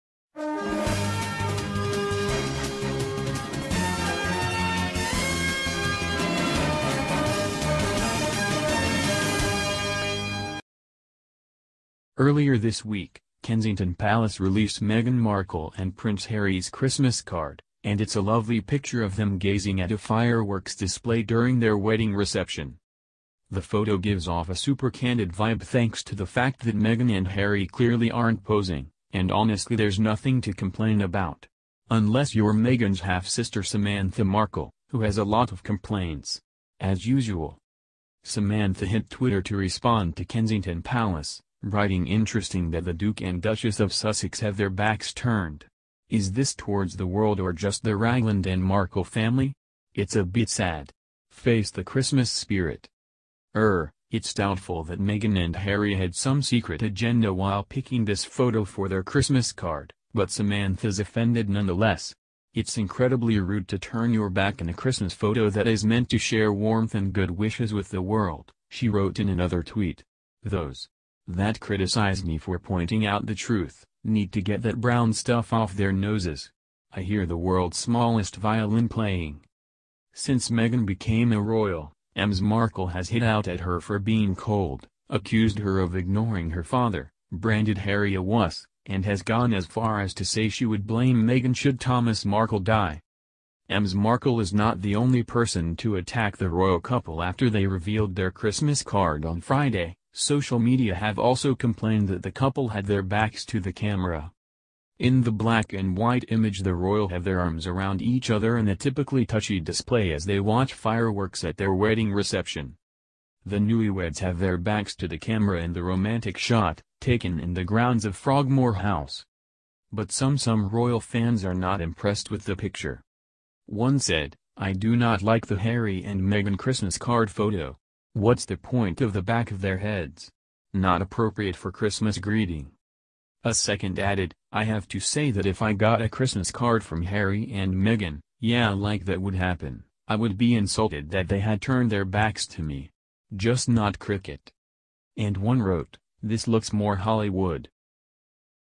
Samantha Markle Claim Slams Meghan and Harry's Christmas Photo for Being "Incredibly Rude"
Samantha Markle Slams Meghan and Harry's Christmas Photo for Being "Incredibly Rude"